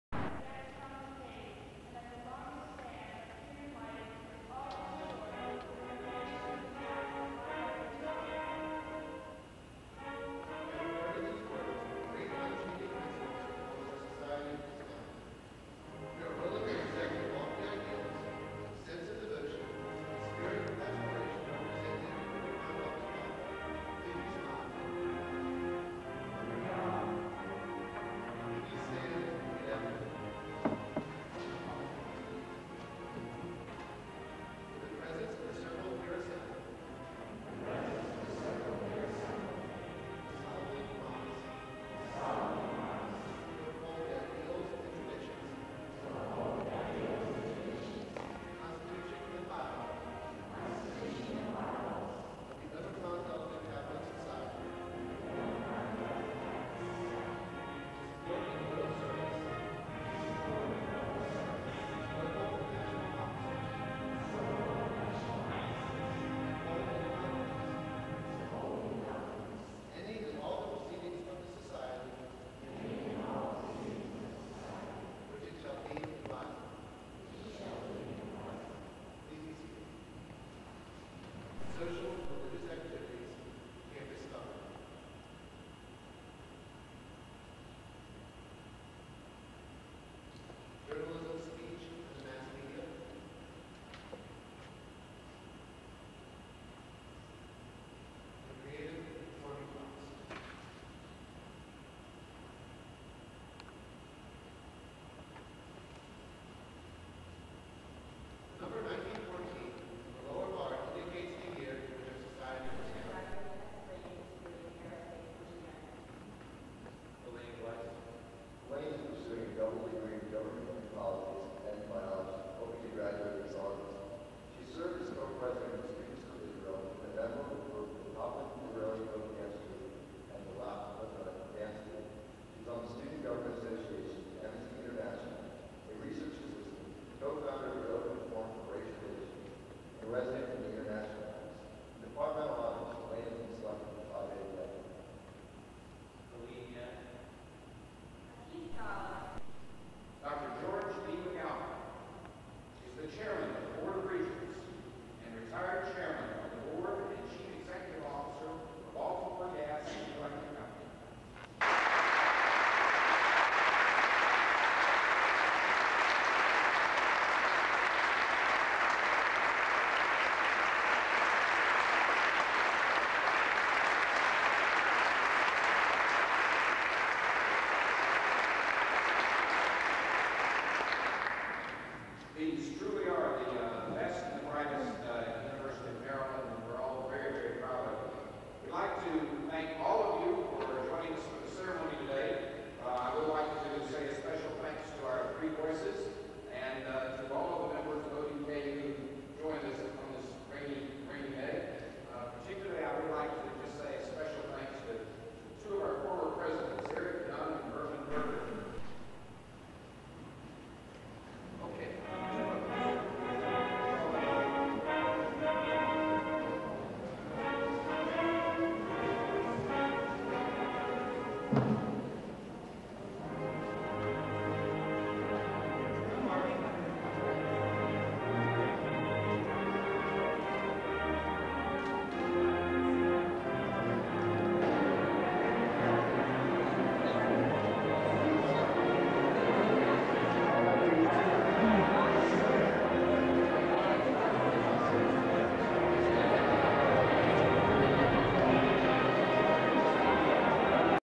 Phi Beta Kappa
Elaine being inducted into the Phi Beta Kappa Society
University of Maryland College Park
April 1994